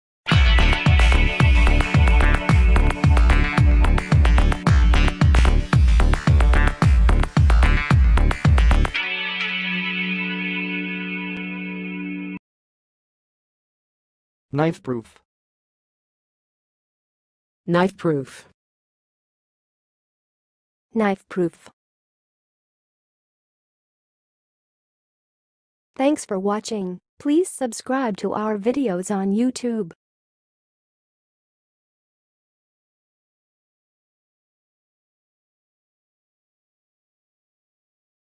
KNIFEPROOF - HOW TO PRONOUNCE KNIFEPROOF?
KNIFEPROOF - Speak like a pro: How to say Knifeproof in 2023

How to pronounce 'knifeproof'?
What are alternative ways to say
Definition:
Knifeproof refers to something that is resistant to being cut or penetrated by a knife.
It can describe materials or products that have been designed to withstand knife attacks or prevent knife injuries.
Pronunciation:
To pronounce 'knifeproof', say 'nahyf-proof'.
Alternative variants include 'nife-proof' and 'knife-proof'.
Test yourself by speaking the following examples:
1. The company specializes in manufacturing knifeproof clothing
2. He wore a pair of knifeproof gloves to protect his hands while working
3. This security film can be applied to glass windows to make them knifeproof
4. The jacket is made from tough knifeproof fabric that can withstand sharp objects
5. The knifeproof door is designed to prevent unauthorized entry
Hope it will help you to answer on the following questions:
1. What are some examples of knifeproof materials?
2. How does knifeproof clothing work?
3. Can you recommend any knifeproof backpacks?
4. Are there any knifeproof windows available?
5. Where can I find knifeproof gloves?